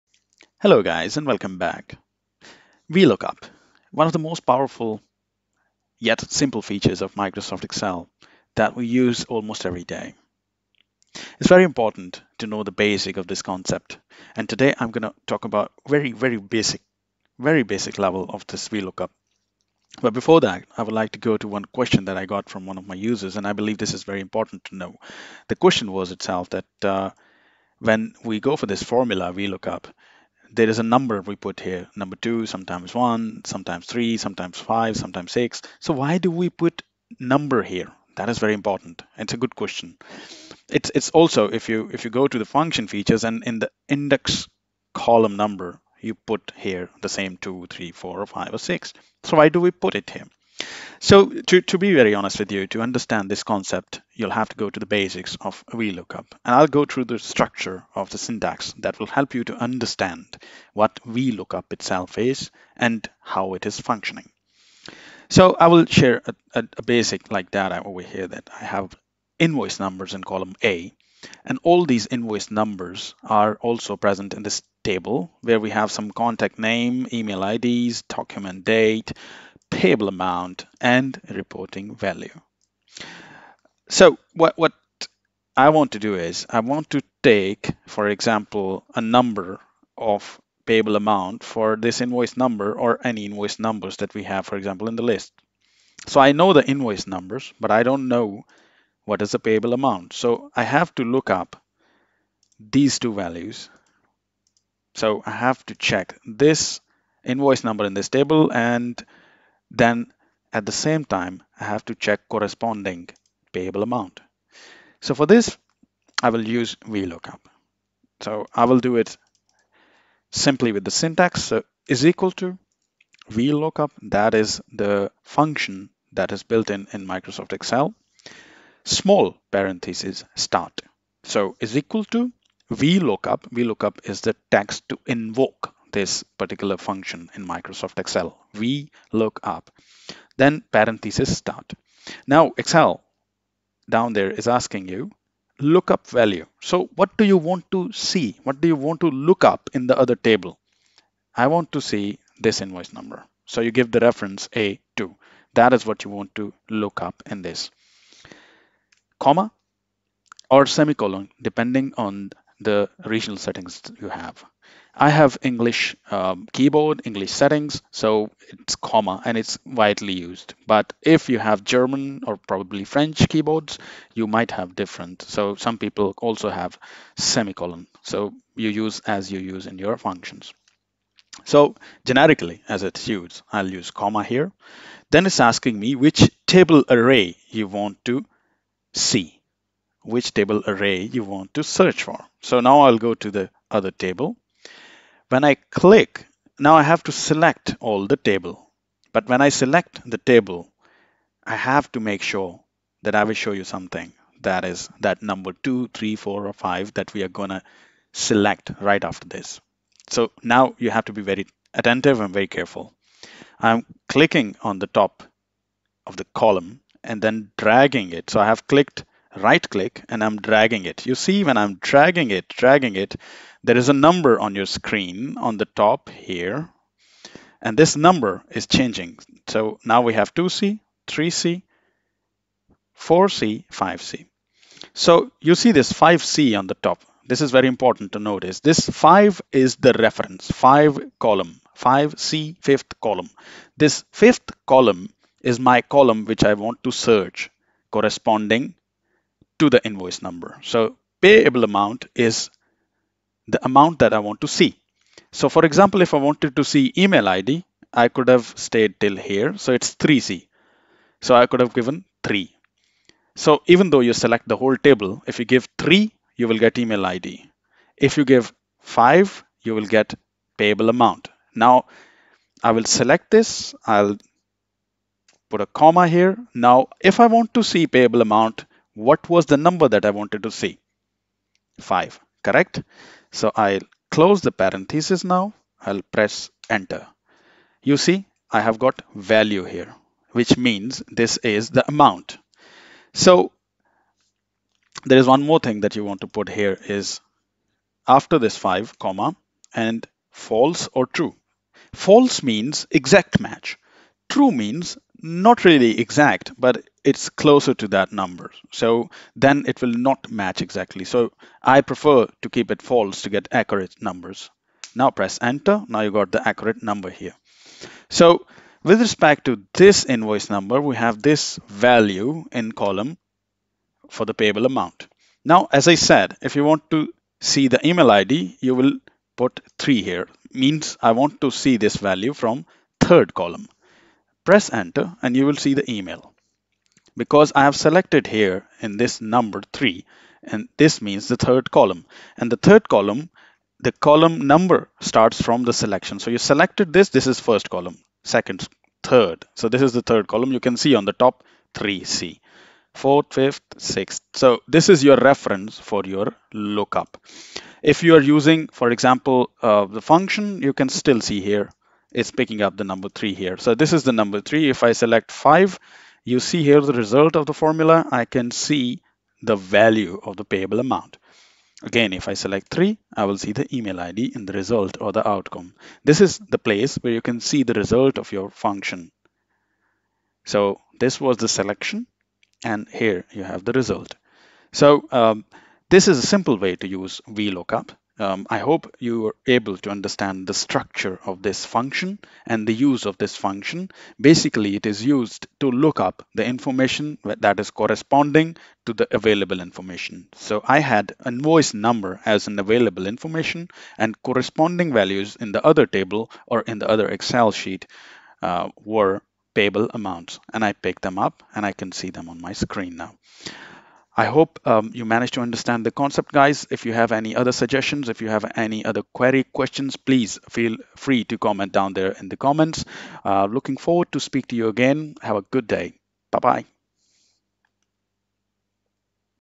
Vlookup simply explained
Simple explanation of vlookup function and made it easy for everyone to learn.